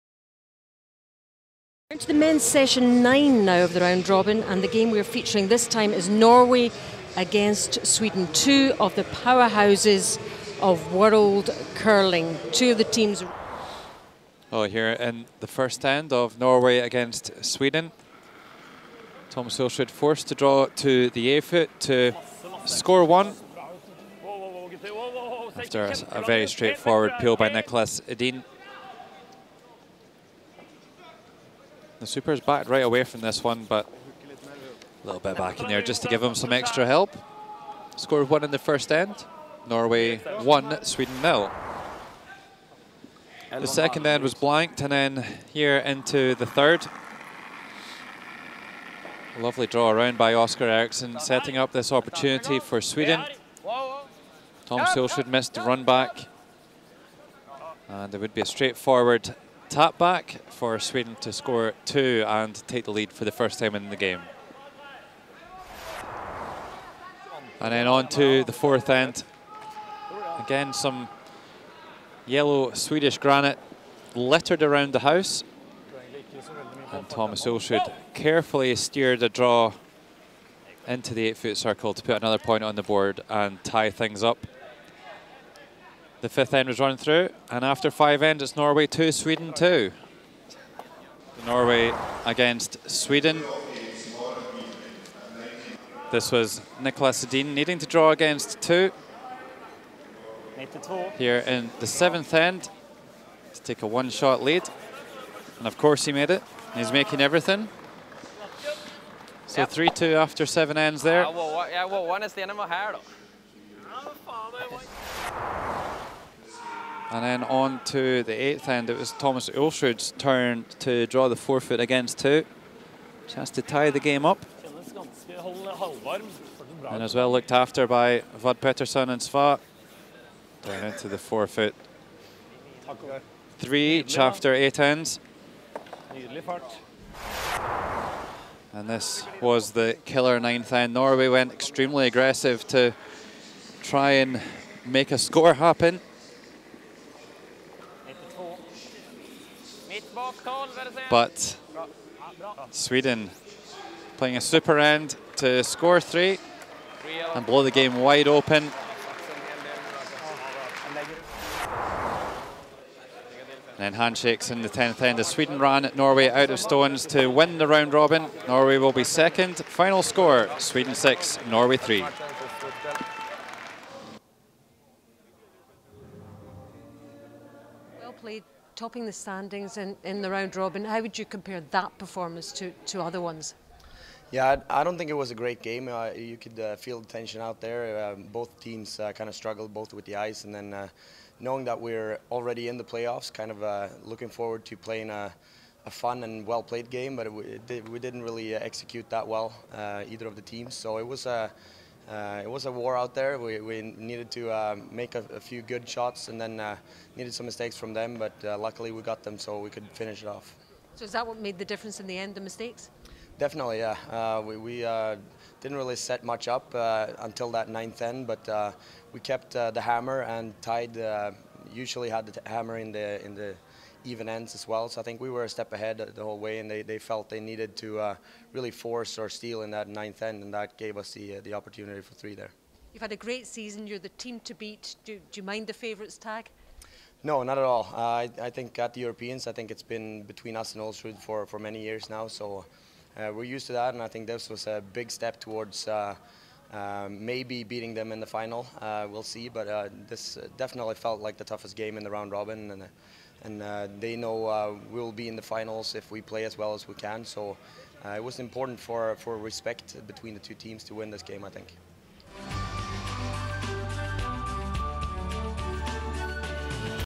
HIGHLIGHTS: Norway v Sweden (Men) - Le Gruyère AOP European Curling Championships 2016
Highlights of the Le Gruyère AOP European Curling Championships 2016, men's round-robin game - Norway v Sweden at the intu Braehead Arena.

All information about this competition can be found at the event